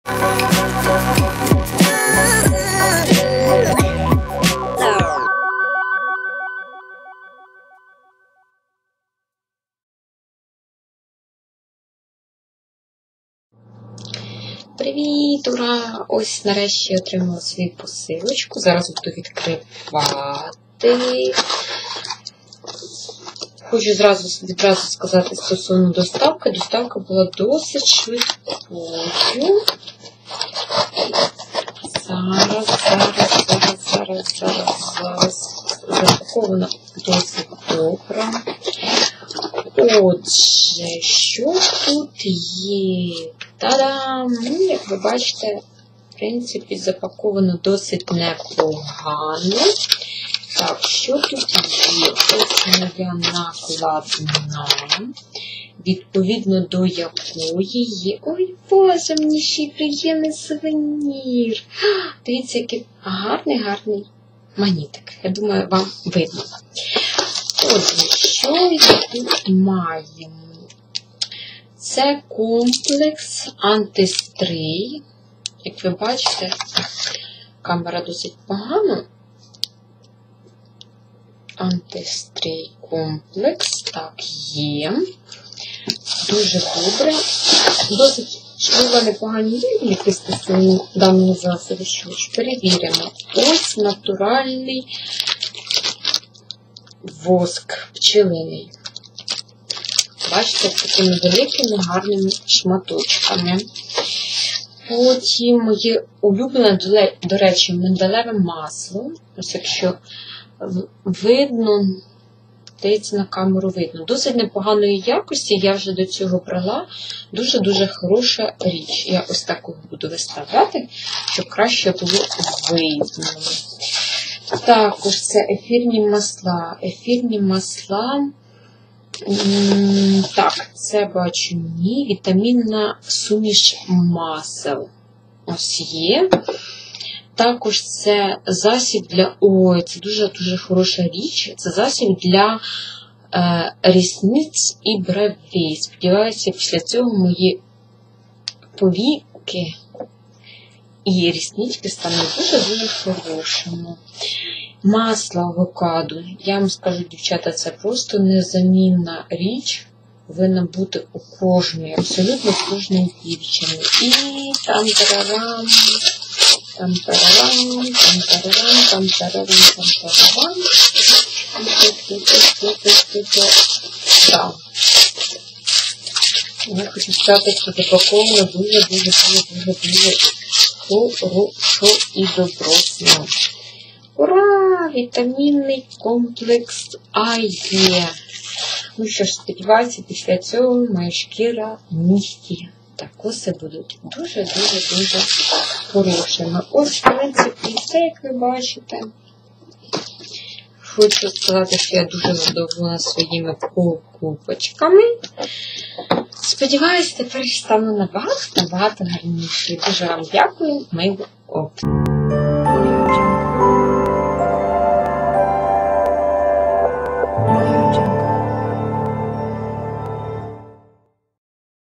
Масла и воски для мыловарения// Все натуральное для косметики
Наш постоянный покупатель заказал на нашем сайте мыло-опт.com.ua и снял свое небольшое видео.

Поддержим еe лайками и подпиской)))

Не пропустите!